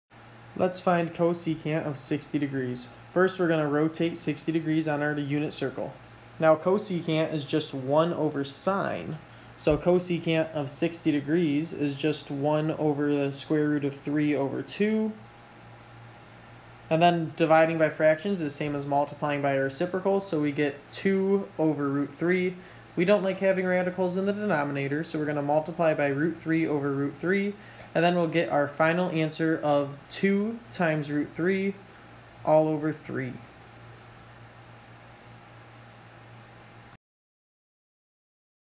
Trigonometry: Find csc (60°)
Marek from SVSU Micro Math helps you evaluate cosecant of an angle by using the unit circle.  The angle is given in degree measure.

Problem: Find csc (60°)